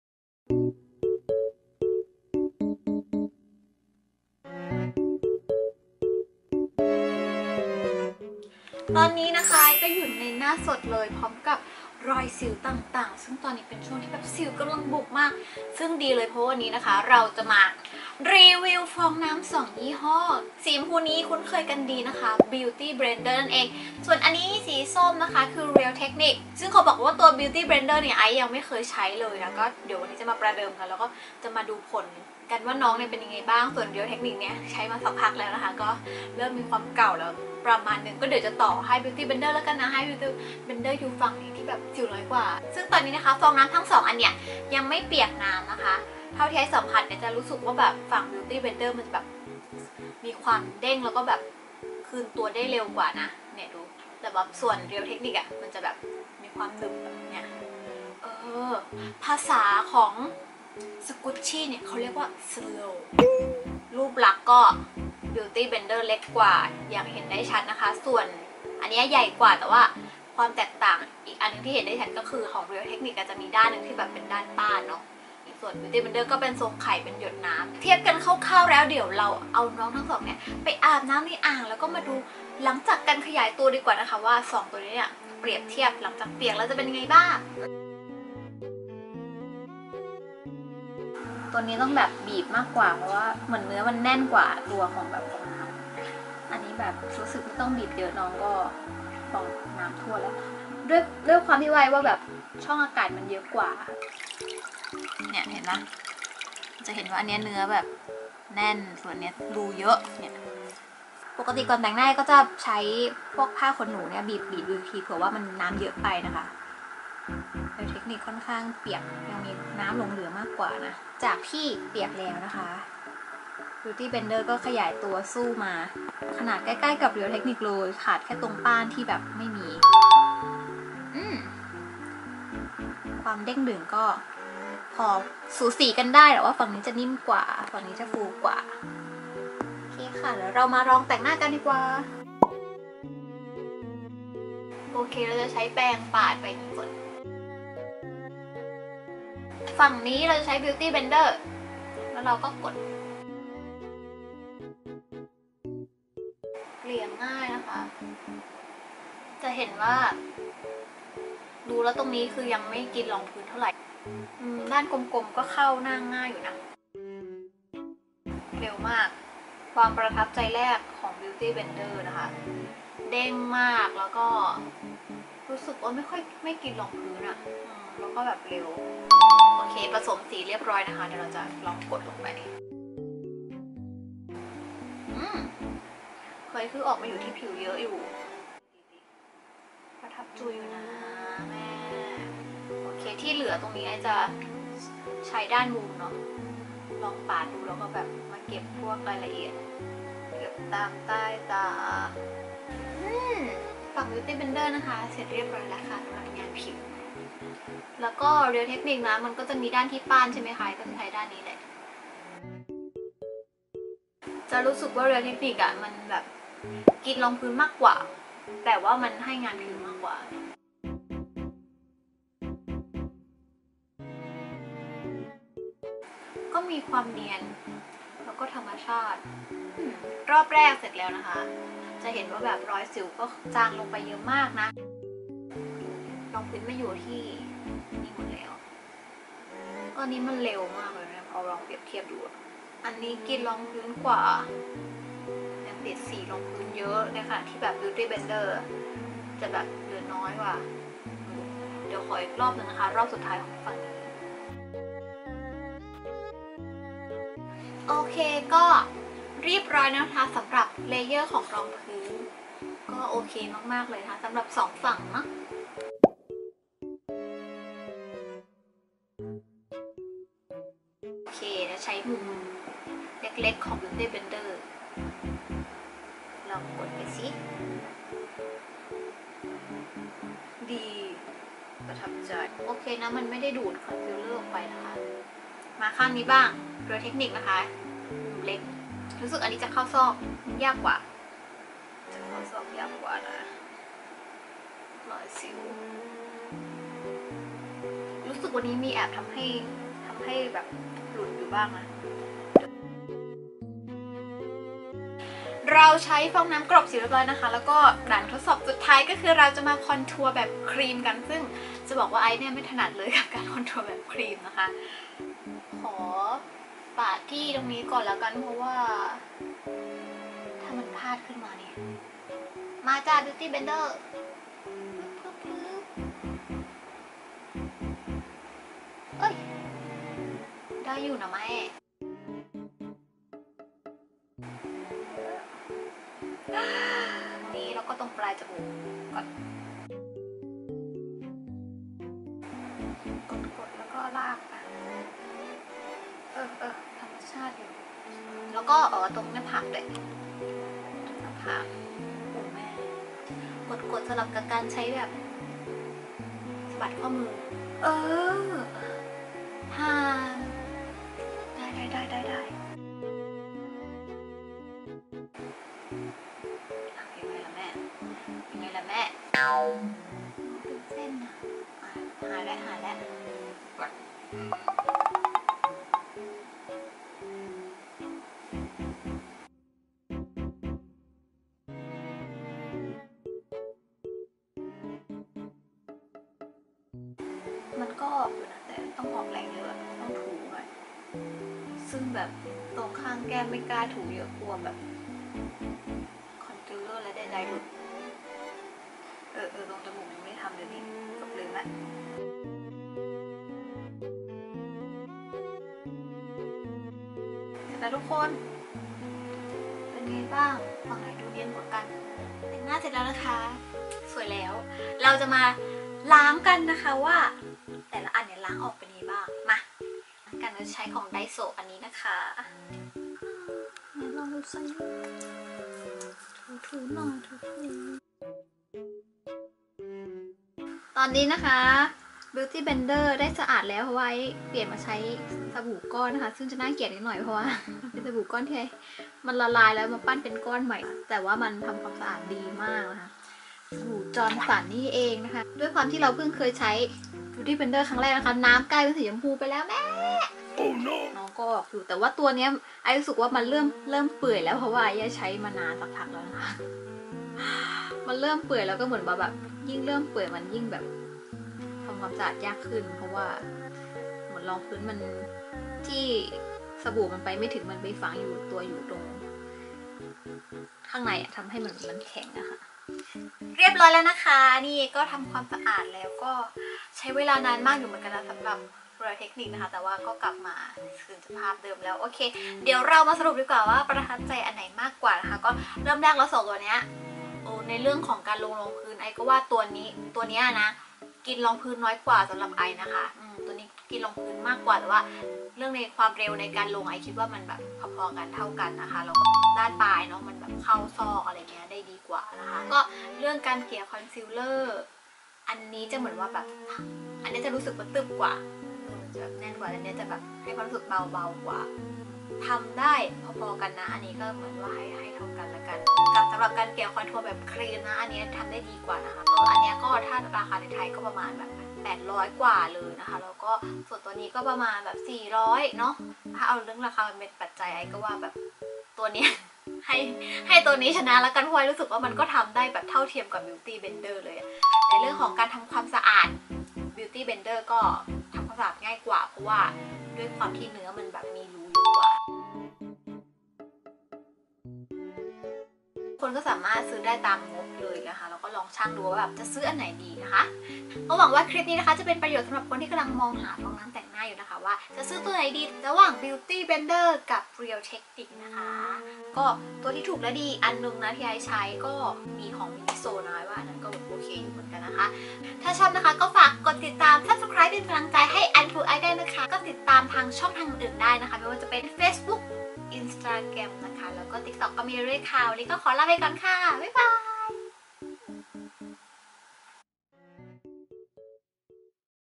รีวิวฟองน้ำไข่เทียบ2 ยี่ห้อดังอันไหนดีกว่ากัน! ฟองน้ำ800 บาทคุ้มไหม? - Unfull Ice
รีวิวฟองน้ำแต่งหน้าแบรนด์ดัง
ฟองน้ำไข่ Beautyblender
ฟองน้ำ Real Techniques
ฟองน้ําแต่งหน้ายี่ห้อไหนดี